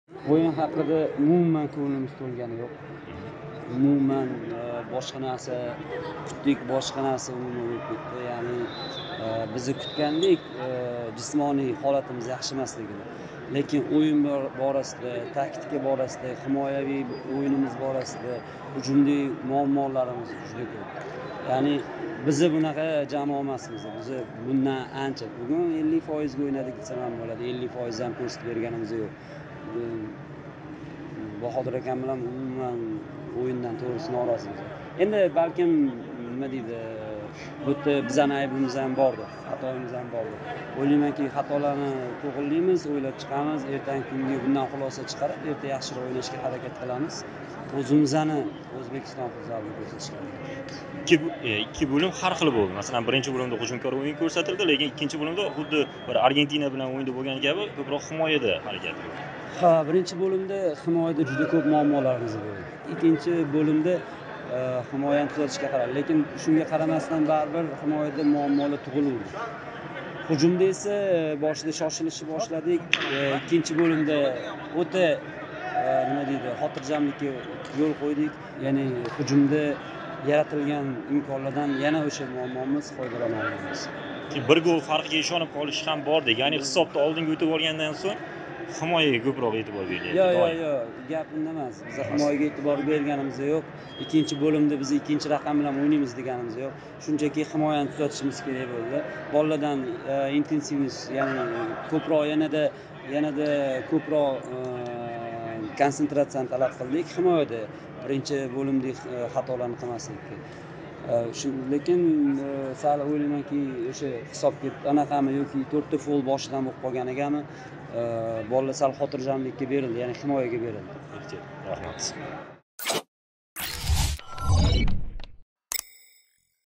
Nodir Eliboyev: «O‘yindan ko‘nglimiz to‘lmadi. Kuchimizning 50 fozini ham ko‘rsatib bera olmadik»
JCHga tayyorgarlik ko'rayotgan futzal bo'yicha O'zbekiston milliy jamoasi Janubiy Amerikaning kuchli jamoalaridan biri Venesuela bilan o'rtoqlik o'yini o'tkazdi. Bellashuvda 3:3 hisobidagi jangovar durang qayd etildi. O'yindan keyin milliy jamoamiz murabbiyi Nodir Eliboyev bilan suhbatlashdik.